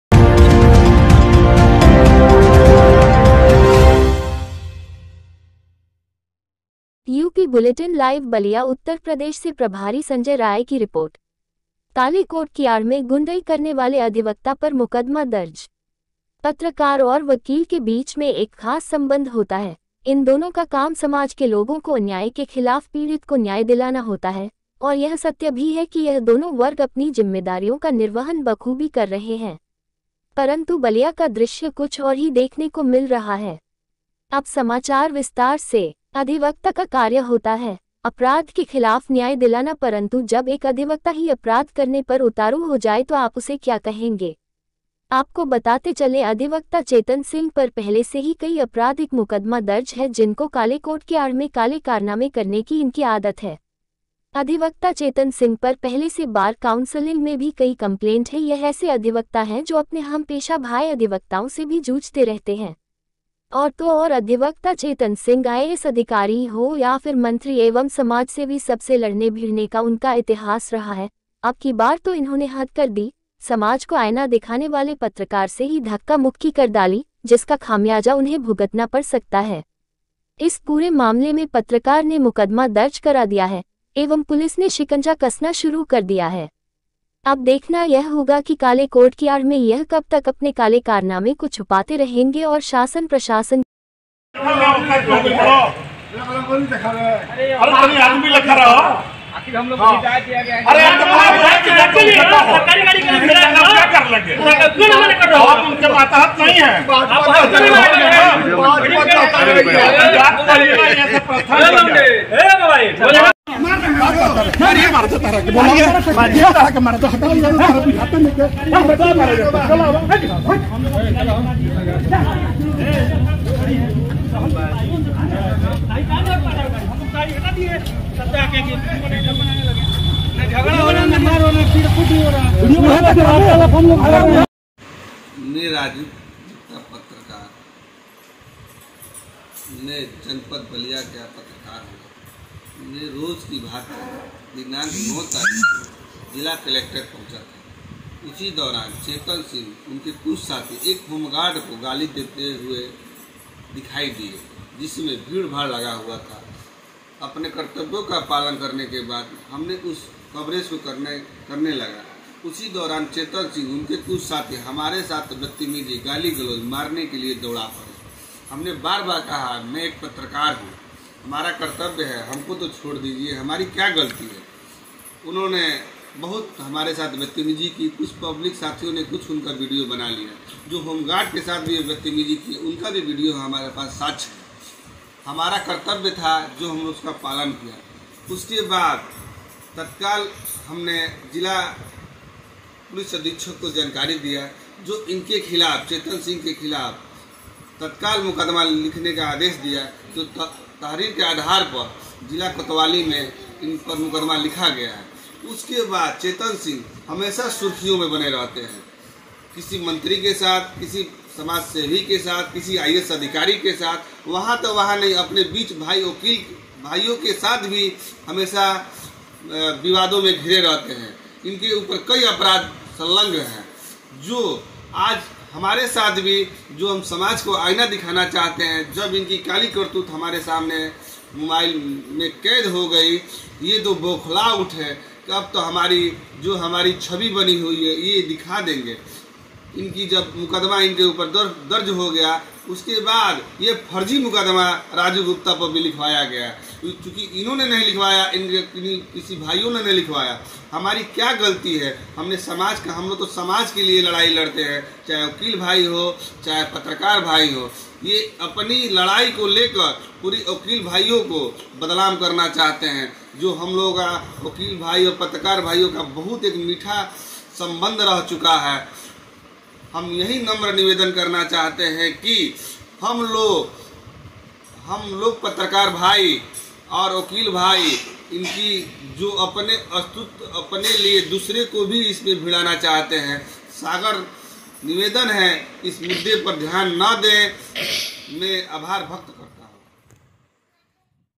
काले कोट की आड़ में गुंडई करने वाले अधिवक्ता पर मुकदमा दर्ज।
*स्थान - बलिया उत्तर प्रदेश*

 *रिपोर्ट - संजय राय*

 *स्लग :- काले कोट की आड़ में गुंडई* *करने वाले अधिवक्ता पर* *मुकदमा दर्ज।*

 *एंकर :-*
पत्रकार और वकील के बीच में एक खास संबंध होता है इन दोनों का काम होता है अन्याय के खिलाफ पीड़ित को न्याय दिलाना।  परंतु बलिया का दृश्य कुछ और ही देखने को मिल रहा है पिछले दिनों  एक वकील पत्रकार के ऊपर रौब झाड़ते नजर आया।

वीओ :- दरअसल पूरा मामला बलिया कलेक्ट्रेट पर हुआ जहां वकील चेतन सिंह होमगार्ड्स पर बेवजह अपना रौब झाड़ते नजर आए जिसको लेकर काफी भीड़ इकट्ठा हो गई थी। सूचना मिलते ही पत्रकार राजू गुप्ता कवरेज करने वहां पहुंच गए कवरेज करने के दौरान ही वकील चेतन सिंह एवं उनके साथी पत्रकार राजू गुप्ता से उलझ गए। धक्का-मुक्की करने लगे पत्रकार राजू गुप्ता ने इसकी सूचना तत्काल पुलिस अधीक्षक को दी एवं पूरी घटना की लिखित तहरीर दी। इस मामले में पुलिस अधीक्षक ने तत्काल तहरीर के आधार  पर मुकदमा दर्ज कर वकील चेतन सिंह पर शिकंजा कसना प्रारंभ कर दिया है।

एंकर :- अधिवक्ता का कार्य होता है अपराध के खिलाफ न्याय दिलाना परंतु जब एक अधिवक्ता ही अपराध करने पर उतारू हो जाए तो आप उसे क्या कहेंगे।

वीओ :- आपको बताते चलें अधिवक्ता चेतन सिंह पर पहले से ही कई अपराधिक मुकदमा दर्ज है जिनको काले कोट के आड़ में काले कारनामे करने की इनकी आदत है ।अधिवक्ता चेतन सिंह पर पहले से बार काउंसलिंग में भी कई कंप्लेंट दर्ज हैं यह ऐसे अधिवक्ता हैं जो अधिवक्ताओं से भी जूझते रहते हैं और तो और अधिवक्ता चेतन सिंह आईएएस अधिकारी हो या फिर मंत्री एवं समाजसेवी सबसे लड़ने भिड़ने का उनका इतिहास रहा है अबकी बार तो इन्होंने हद कर दी समाज को आईना दिखाने वाले पत्रकार से ही धक्का-मुक्की कर डाली जिसका खामियाजा उन्हें भुगतना पड़ेगा। इस  पूरे मामले में पत्रकार ने मुकदमा दर्ज करा दिया है एवं पुलिस ने शिकंजा कसना शुरू कर दिया है ।

एंकर :- अब देखना यह होगा कि काले कोर्ट की आड़ में यह कब तक अपने काले कारनामे को छुपाते रहेंगे और शासन प्रशासन की गिरफ्त से बचते रहेंगे।

 *बाइट पीड़ित पत्रकार राजू गुप्ता*